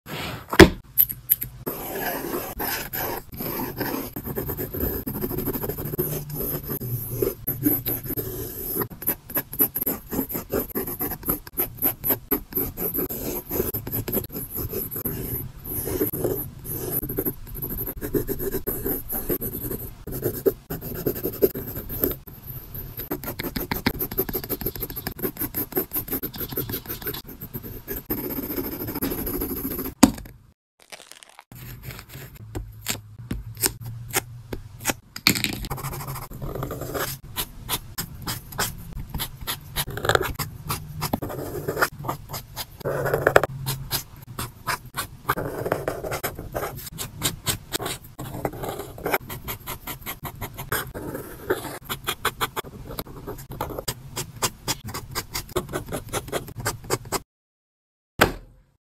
Drawing ASMR🎧 Bonnie! FNAF
Drawing Bonnie from five nights at Freddy's! Drawing ASMR. Satisfying drawing art video! Drawing FNAF Bonnie! Five nights at Freddys drawing satisfying art!